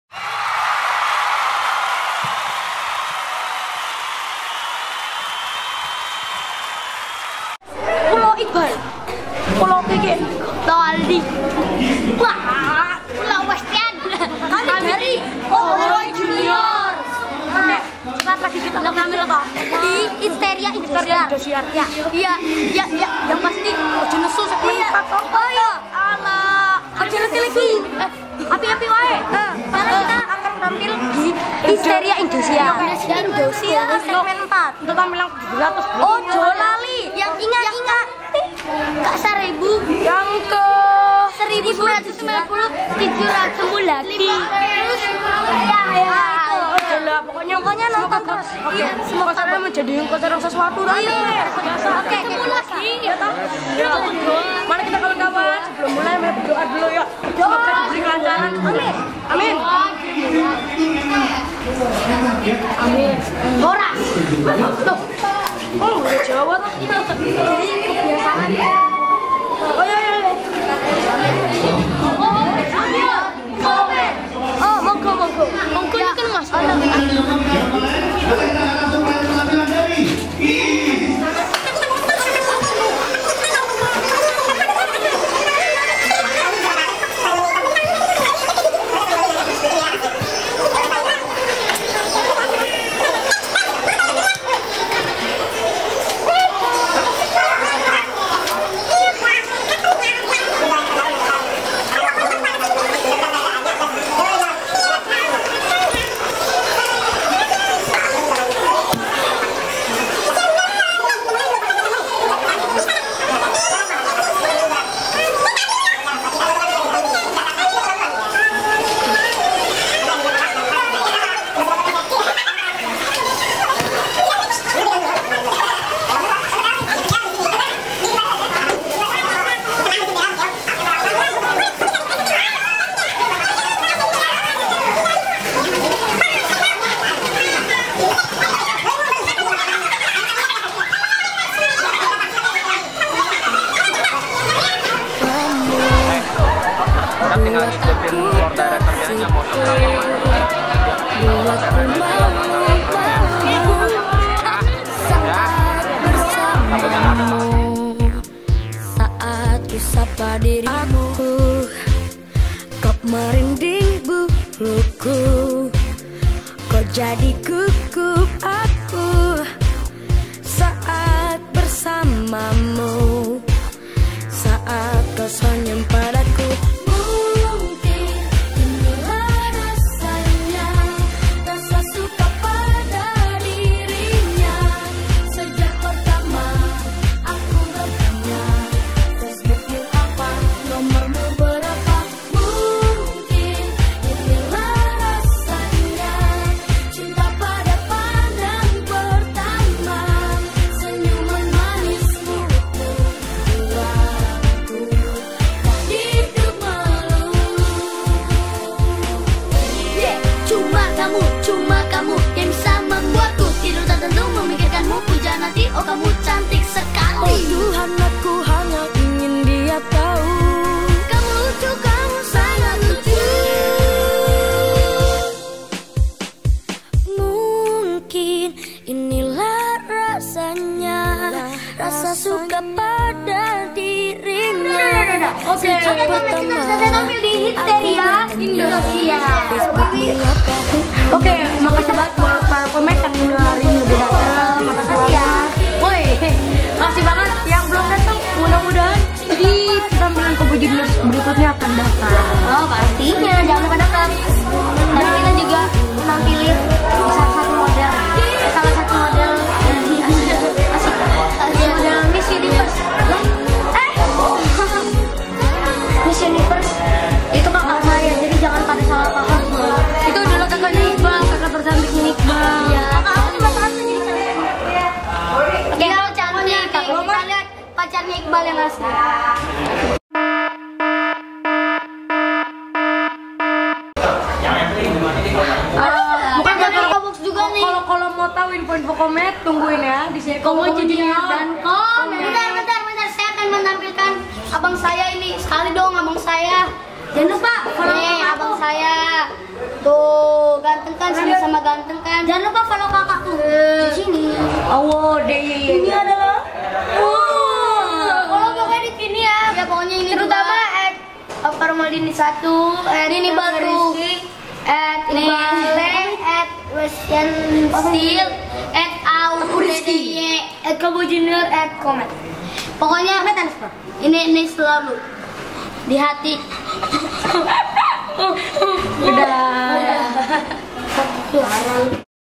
Coboy Junior Hitzteria 26 November 2011 - Behind The Stage
Coboy Jr. Hitzteria Indosiar 26 November 2011 - Behind The Stage

Management go Follow : @INBEK | @inbekplus

Movie and Edited by @ikhsansandy